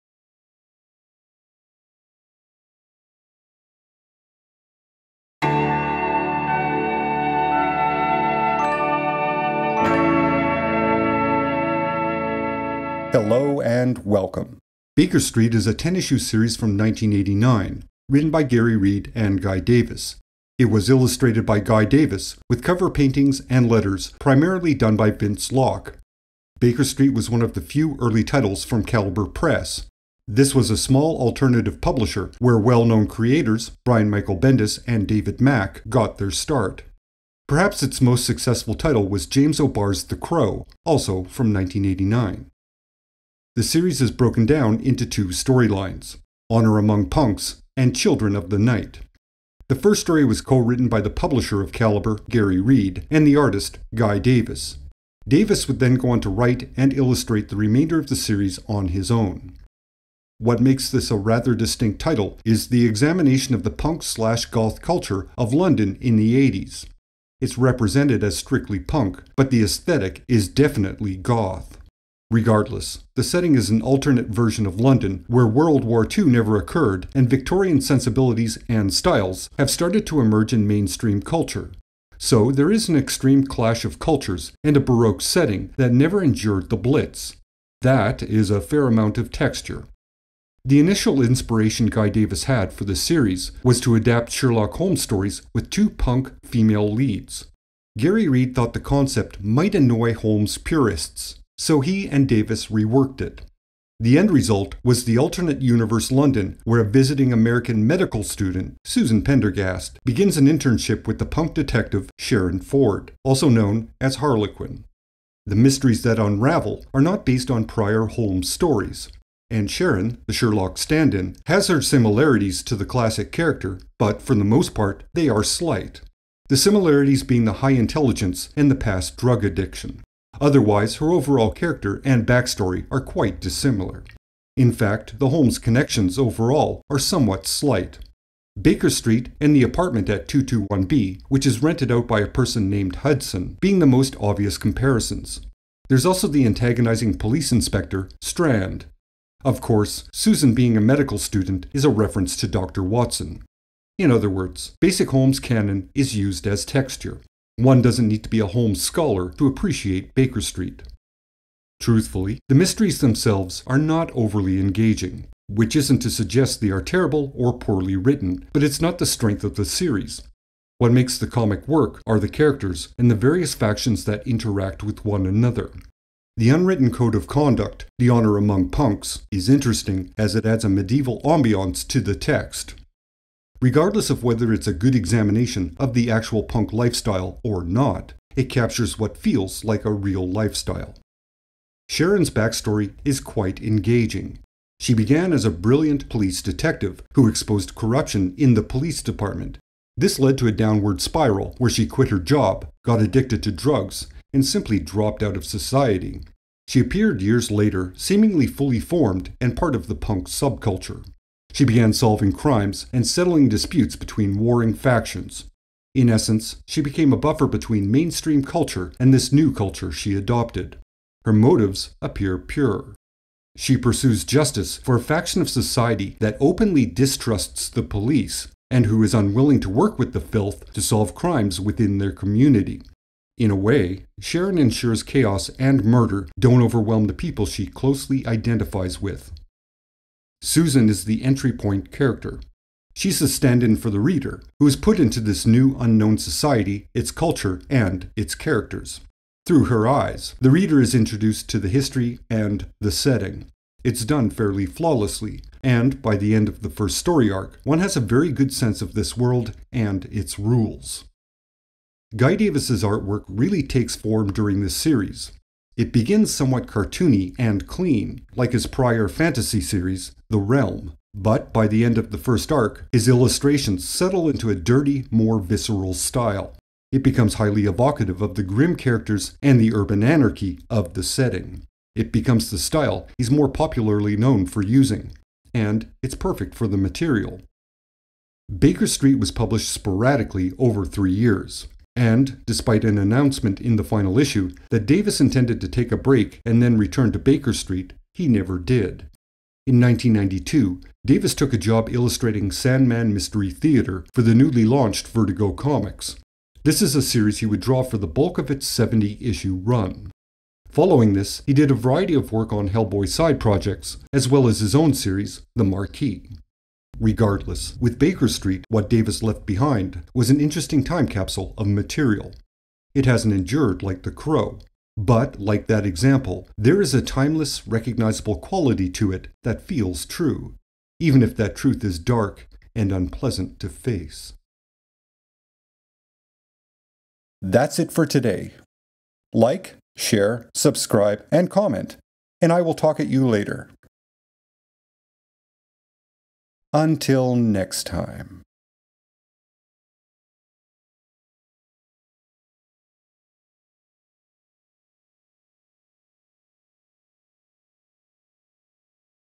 The 80’s Vault: Baker Street
A look at the ten issue series, Baker Street, published by Caliber Press between 1989 and 1992. Written by Gary Reed and Guy Davis. Illustrated by Guy Davis. Cover paintings and letters primarily by Vince Locke.